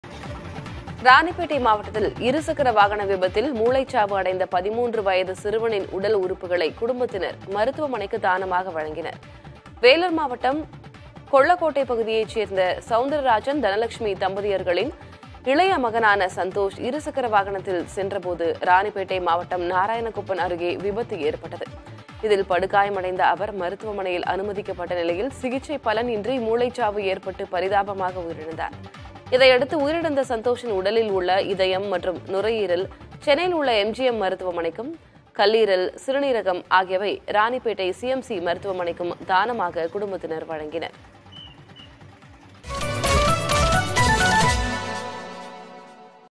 சிறுவன் உயிரிழப்பு: உடல் உறுப்புகளை தானமாக வழங்கிய குடும்பத்தினர் | Vellore | PTT
சிறுவன் உயிரிழப்பு: உடல் உறுப்புகளை தானமாக வழங்கிய குடும்பத்தினர்